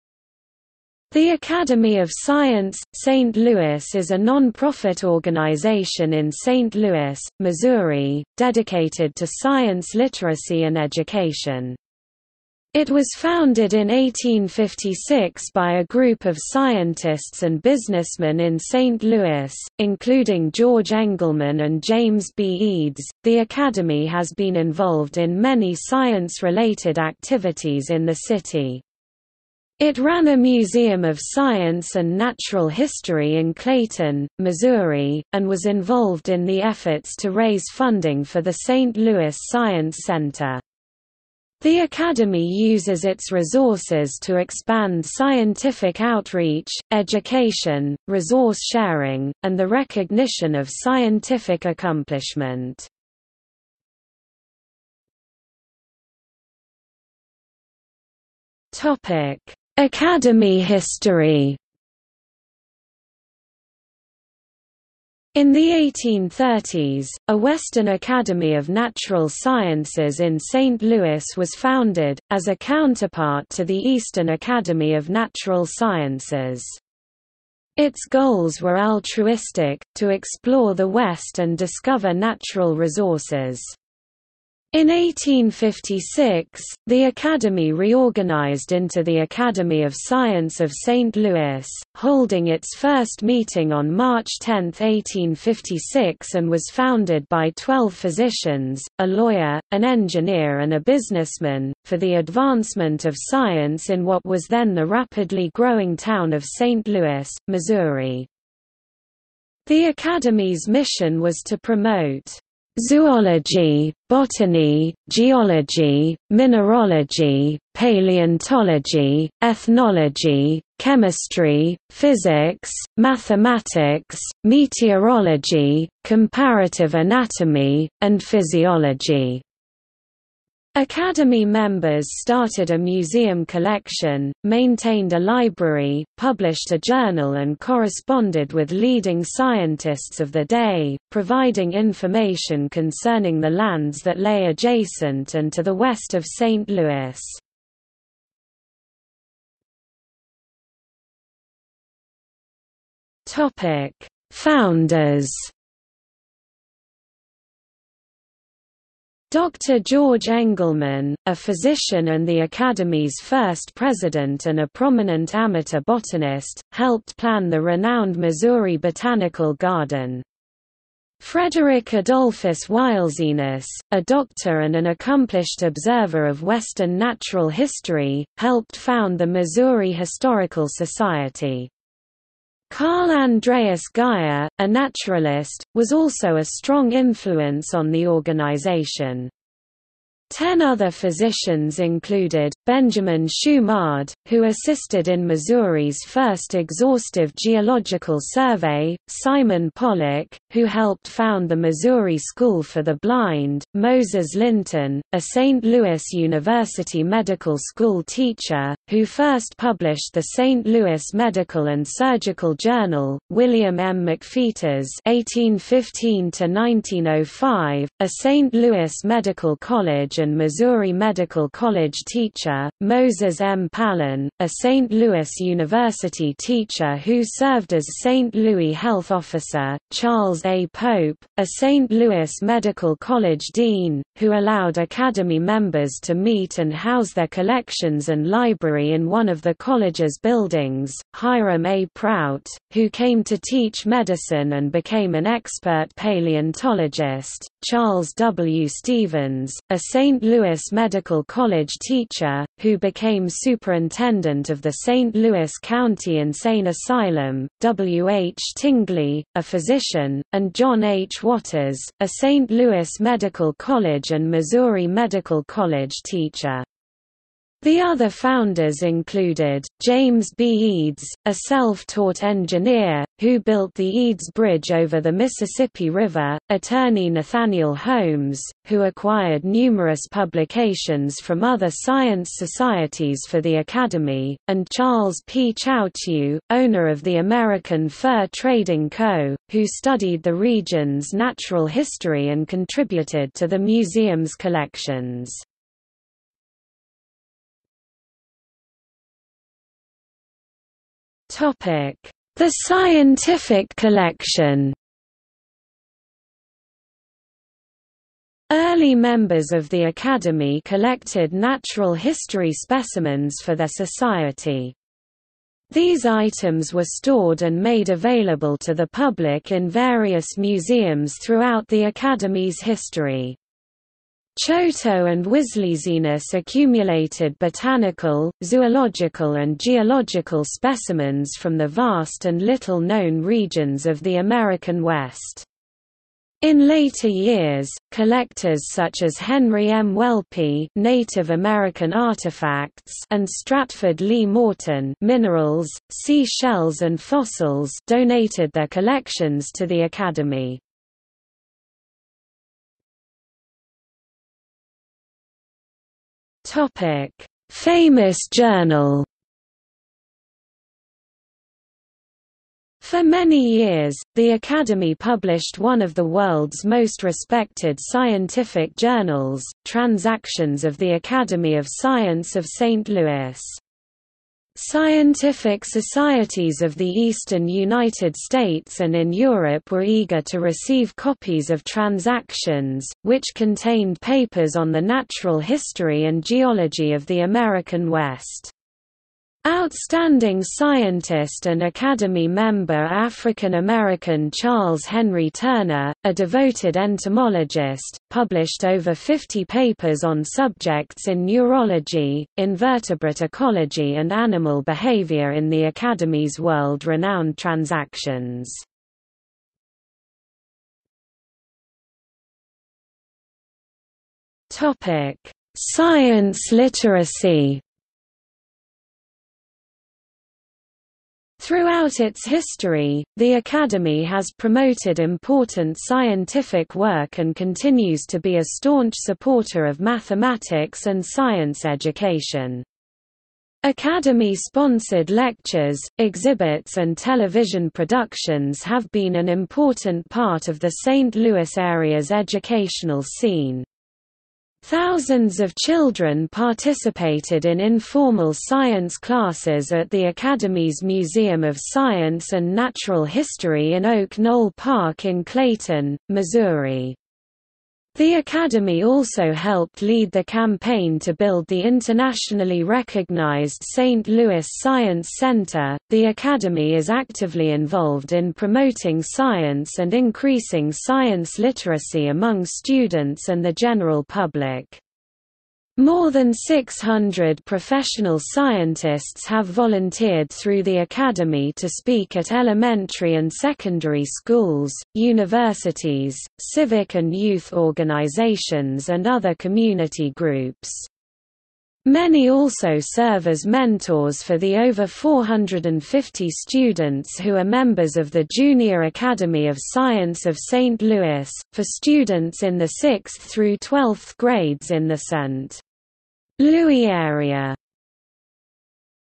Academy of Science, St. Louis | Wikipedia audio article
00:00:55 1 Academy history
00:02:29 2 Founders
00:04:57 3 The Scientific Collection
00:05:52 4 Famous journal
00:06:50 5 Science literacy

- Socrates

SUMMARY
The Academy of Science - St. Louis is a non-profit organization in St. Louis, Missouri, dedicated to science literacy and education. It was founded in 1856 by a group of scientists and businessmen in St. Louis, including George Engelmann and James B. Eads, the Academy has been involved in many science-related activities in the city. It ran a Museum of Science and Natural History in Clayton, Missouri, and was involved in the efforts to raise funding for the St. Louis Science Center. The Academy uses its resources to expand scientific outreach, education, resource sharing, and the recognition of scientific accomplishment.